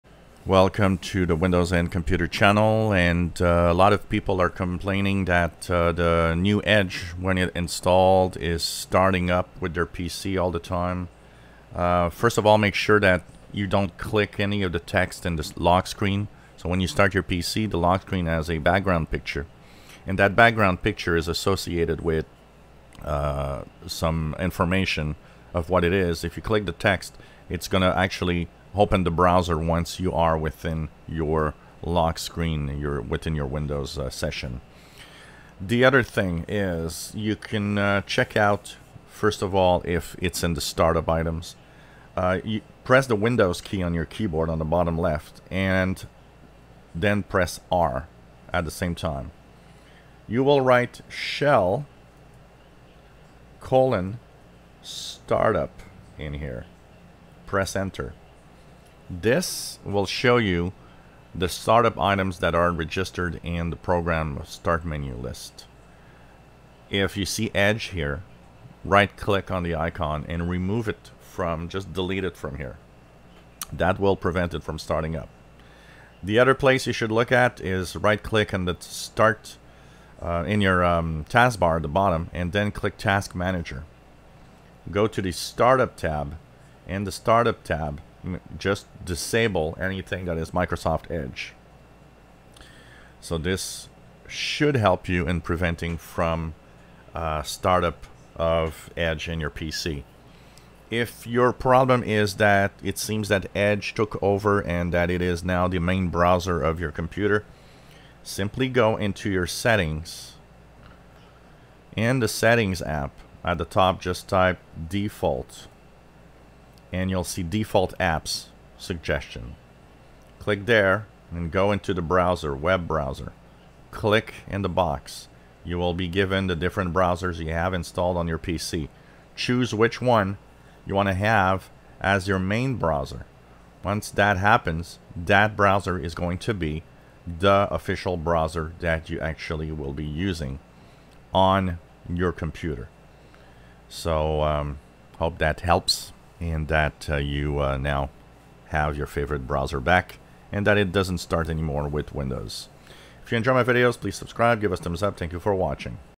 Removing Microsoft Edge at startup and choosing a different default browser July 16th 2020
a few tips to stop Edge from starting up with the PC and how to choose your default browser